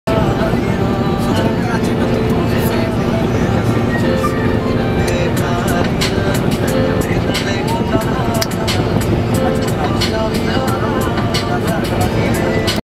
AT Night The Swat Expressway Swat Express Motorway Nowshera–Swat Expressway 12 Oct 2019 KPK Pakistan
AT Night Swat Express Motorway 12 Oct 2019 KPK Pakistan
The Swat Expressway or Nowshera–Swat Expressway is an 81-kilometre-long six-lane expressway in the Khyber Pakhtunkhwa, Pakistan. The expressway stretches from Nowshera to Chakdara in Lower Dir District, passing through Swabi, Mardan, and Malakand Districts.
Length: 81 km
Constructed: May 21, 2018
North end: Chakdara, Lower Dir District
South end: Nowshera